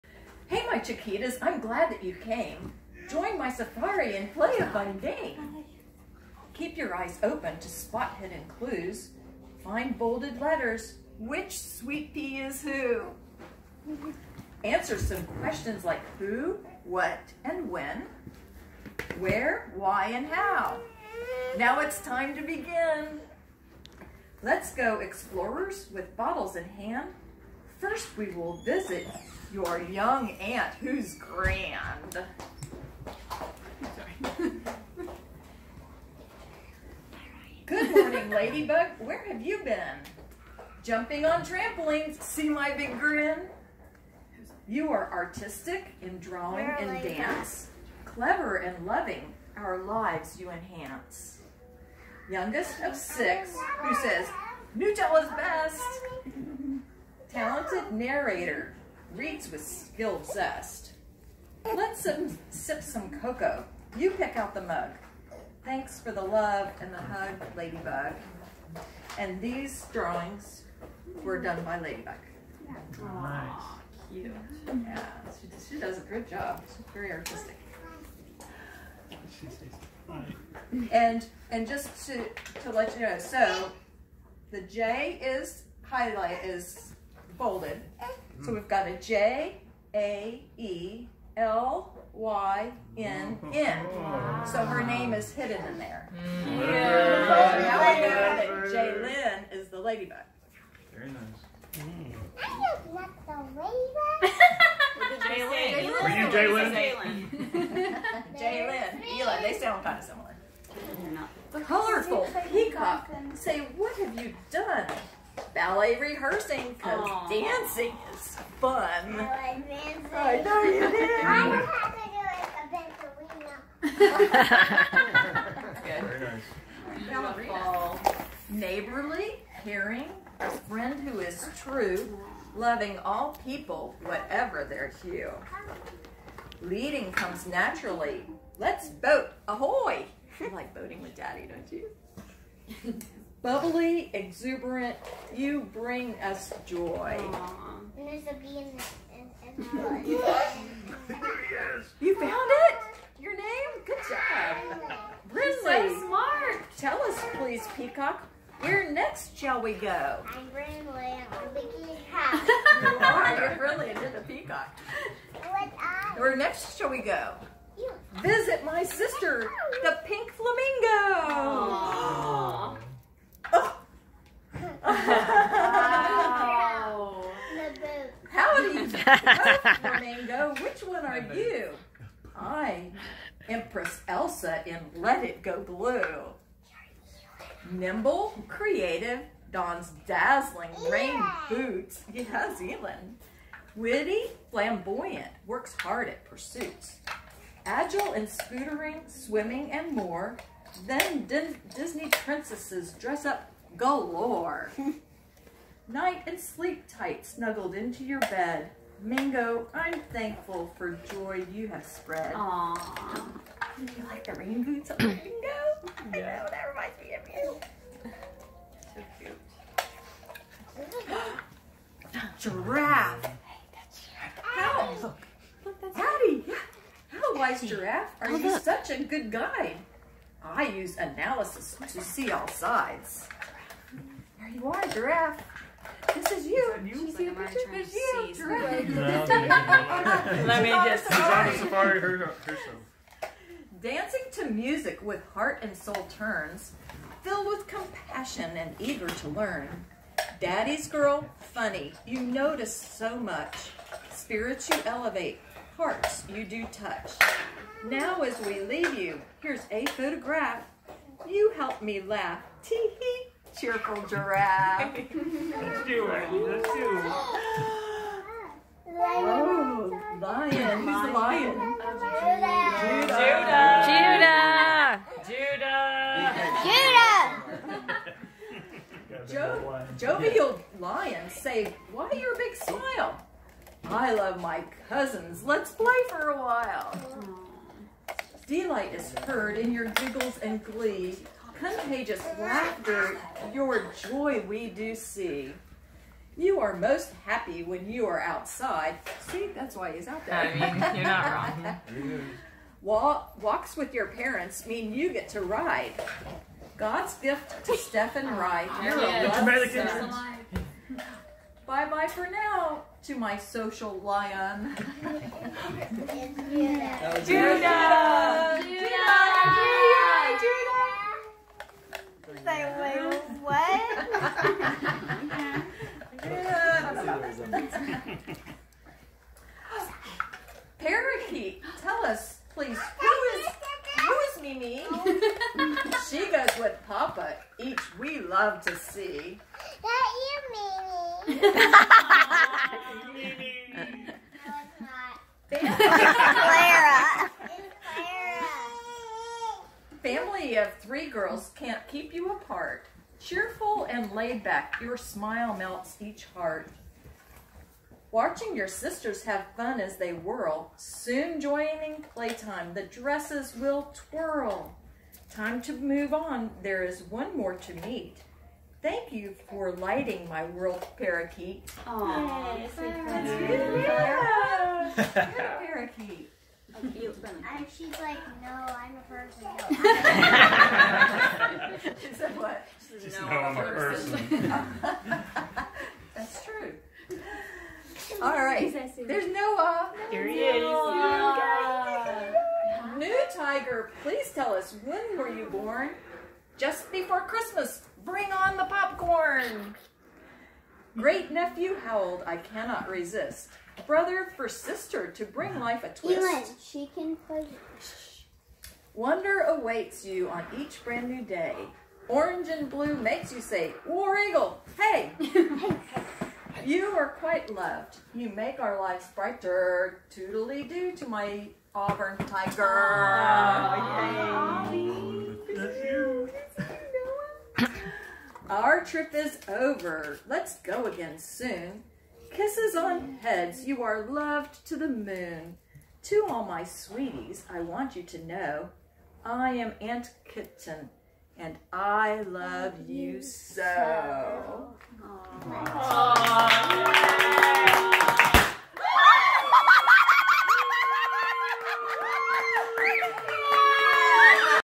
Aunt Kittens Safari Book Reading 4/2021
Aunt Kitten's Safari is Kristin's first book, written for her youngest niece and six great-nieces and nephews. Join the family book launch party as Kristin Emily reads her book to the children.